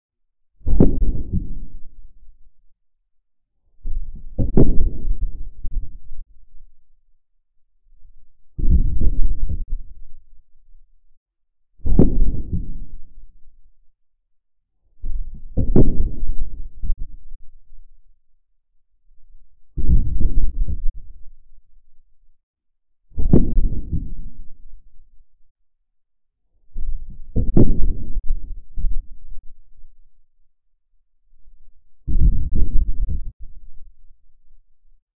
Distant explosions - Sound effects
The sounds of explosions going off in the distance. If you use this sound please link back to this video.